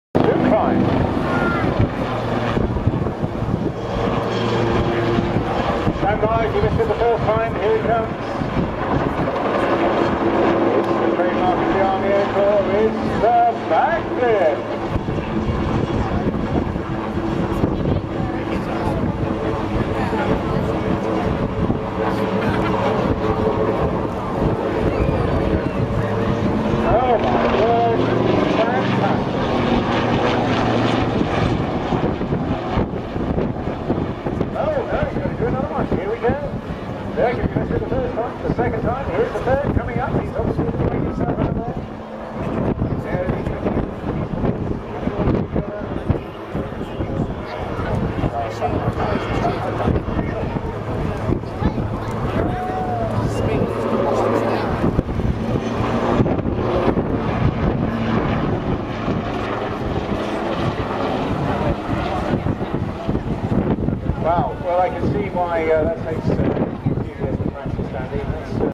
Helicopter Does Double Back Flip. OMG!!
At Eastbourne Airbourne 2012.

Helicopter does two backflips